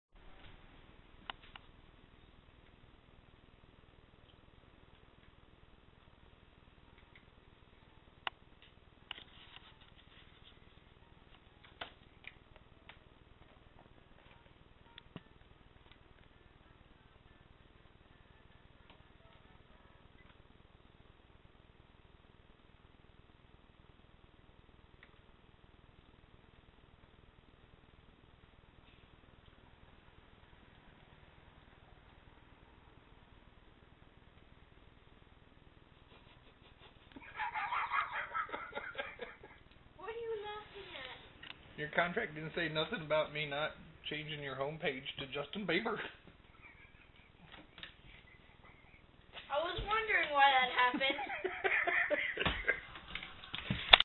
Ian can't stand Justin Bieber
Ian tried to get me to sign a pledge to never mention "JB" so of course I had to change his home page to the Justin Bieber YouTube page.